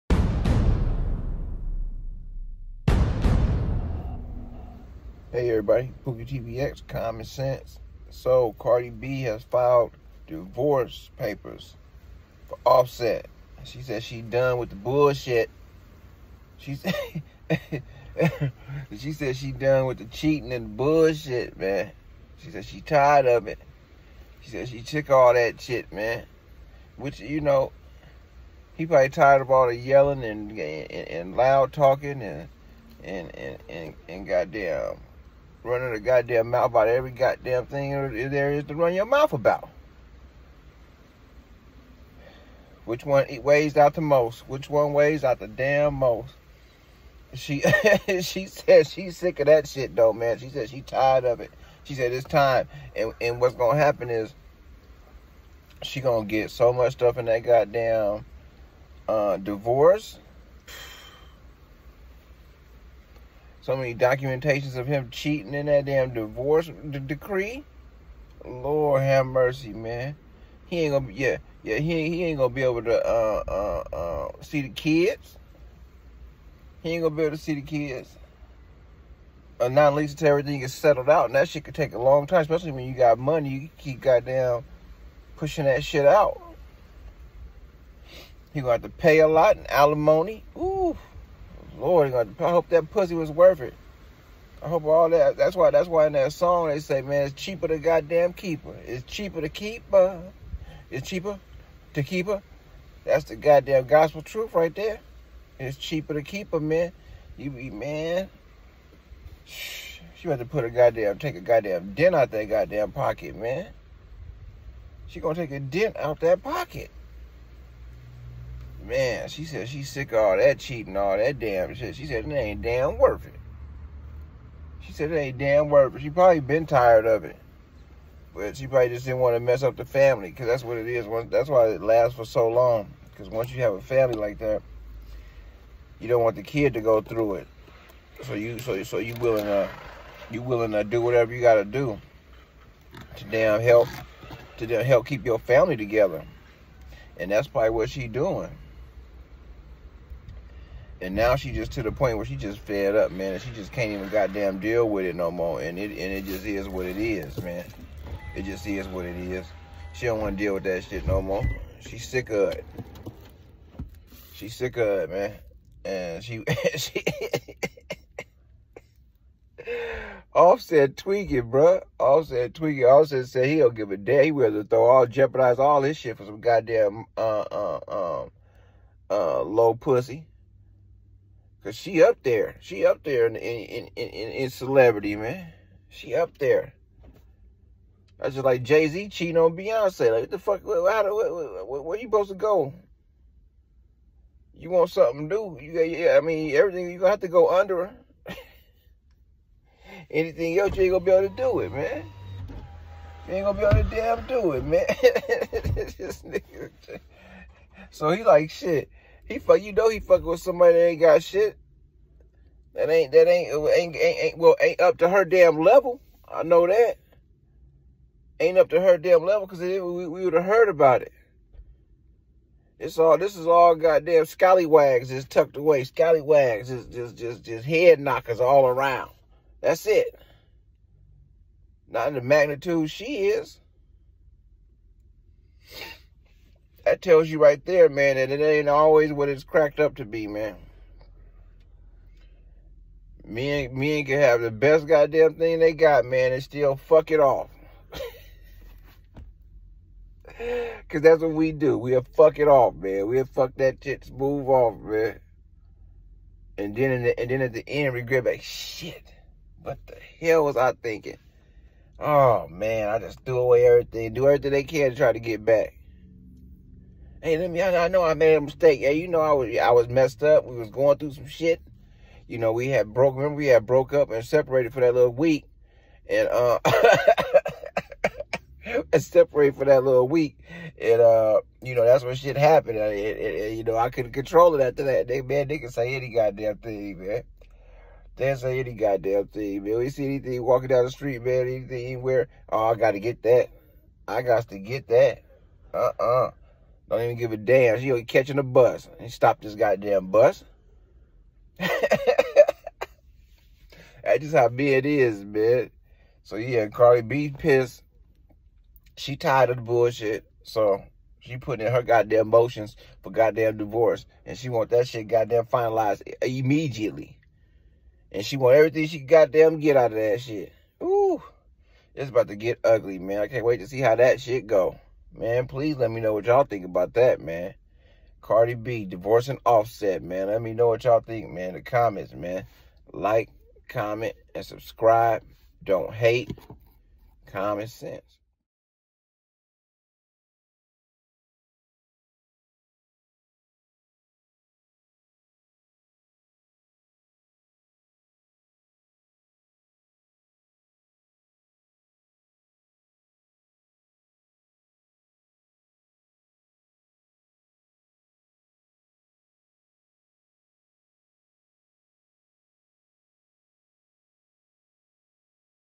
cardib calls it quits with offset and files for a divorce she giving up.
POOKIETVX common sense! cardib calls it quits with offset and files for a divorce she giving up.